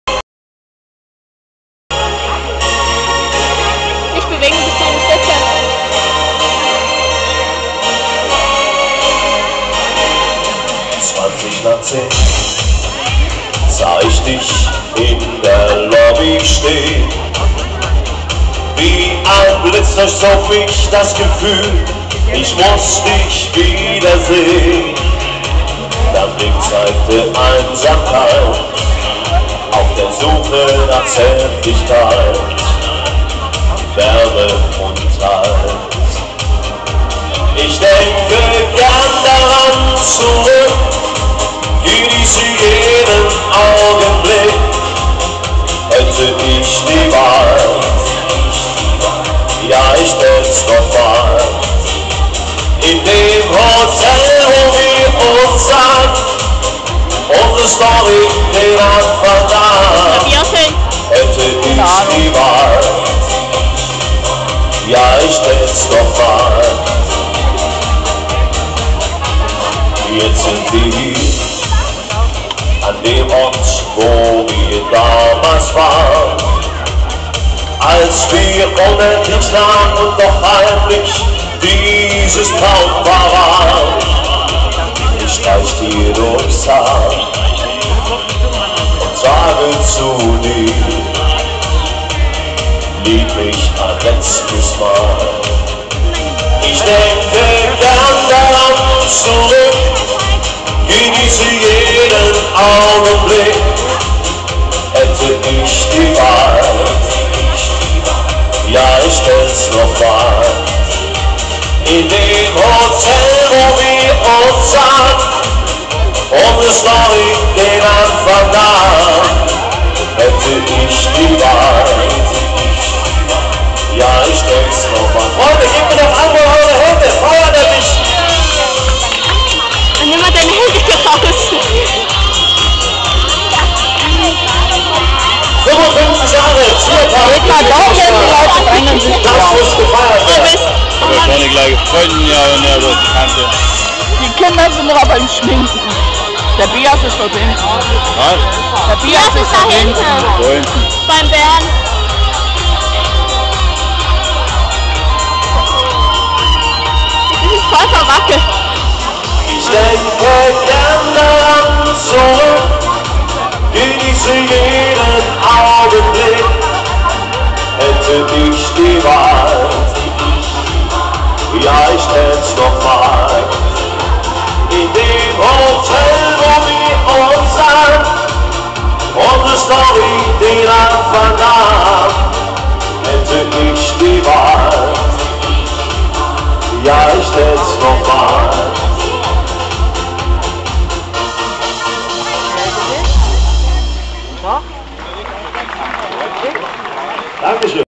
Live im Bischofswerdaer Tierpark (Cover)
Leider weiß ich nicht mehr wem dieser man gecovert hat... Er singt live auf der Bühne im Bischofwerdaer Tierpark.